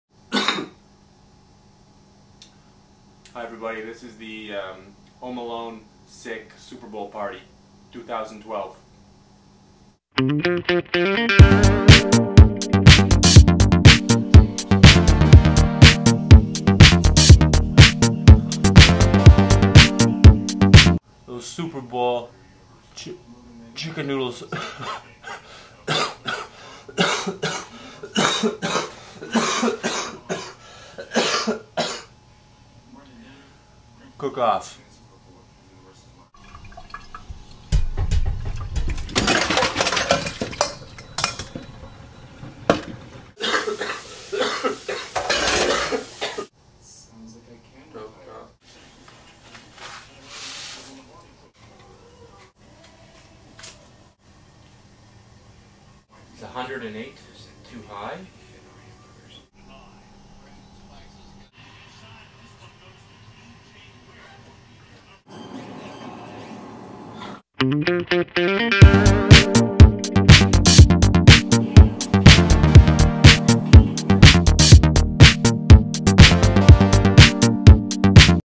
The Home Alone Sick Super Bowl Party 2012
I was home sick on anti biotics for the super bowl.